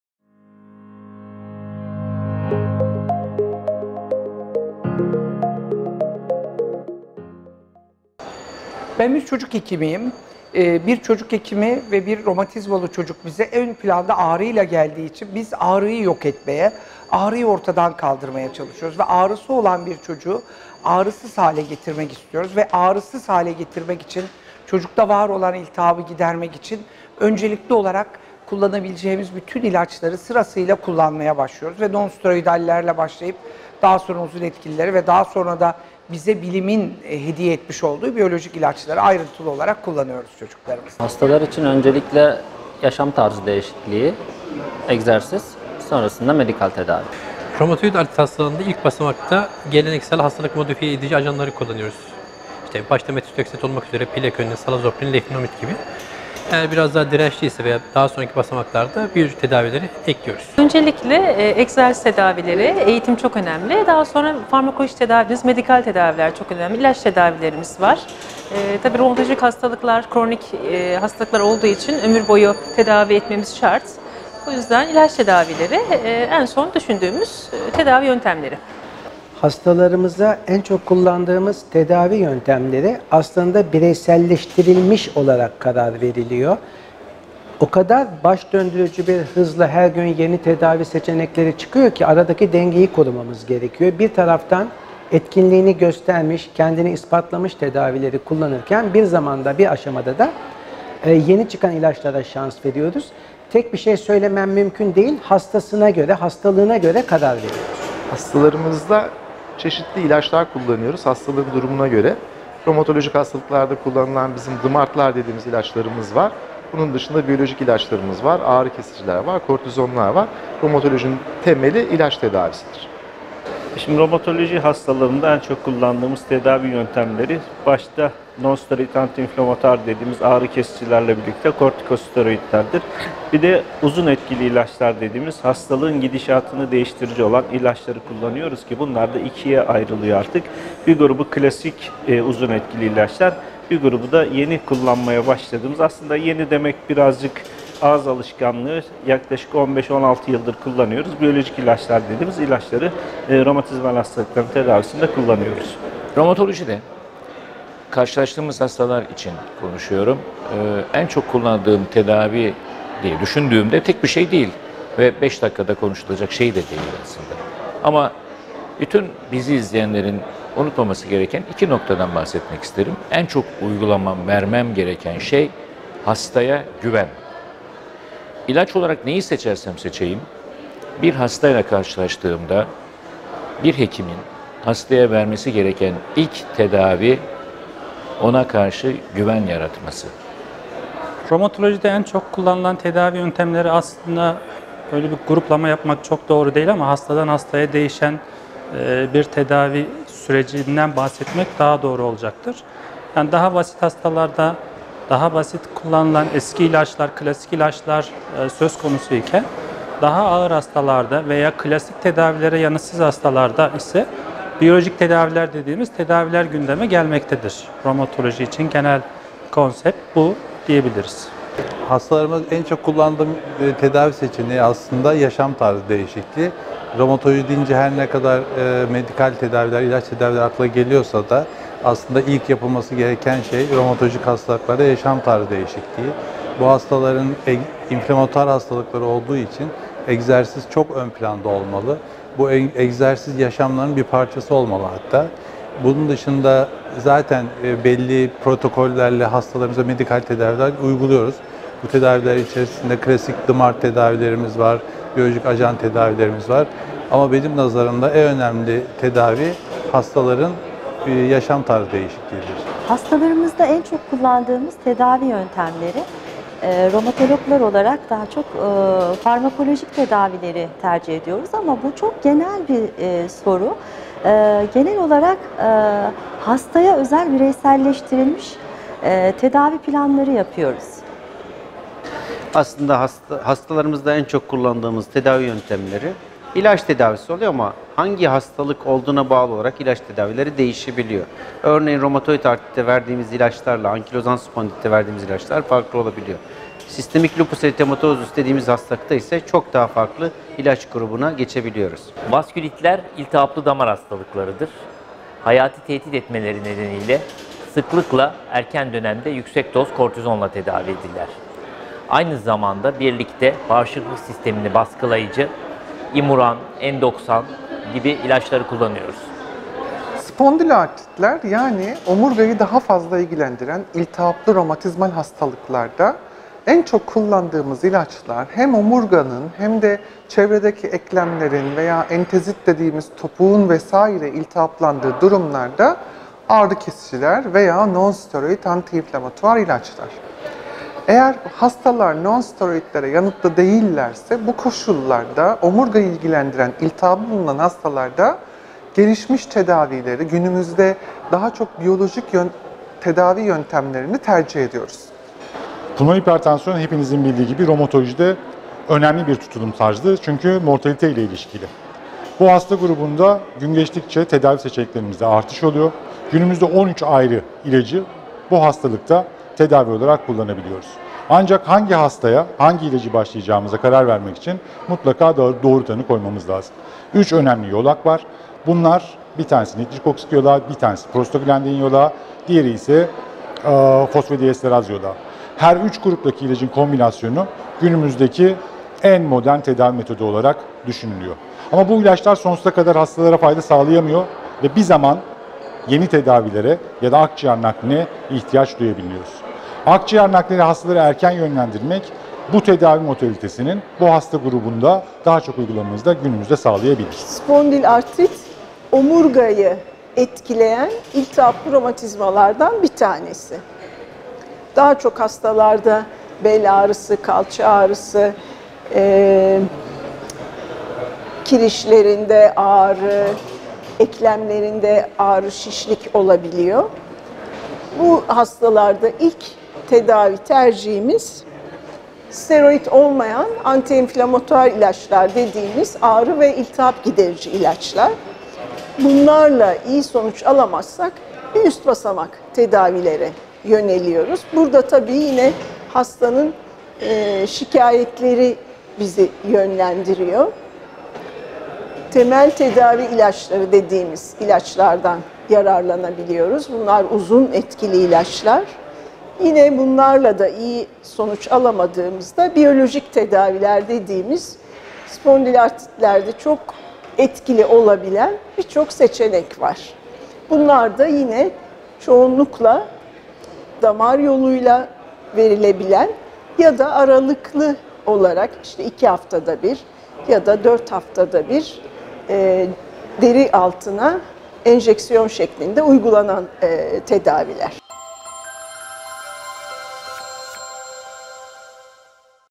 Romatoloji'de uygulanan tedavi yöntemleri nelerdir?
MAVİ PANO'da Romatologlarımıza sorduk...
"Romatoloji'de uyguladığınız tedavi yöntemleri nelerdir?"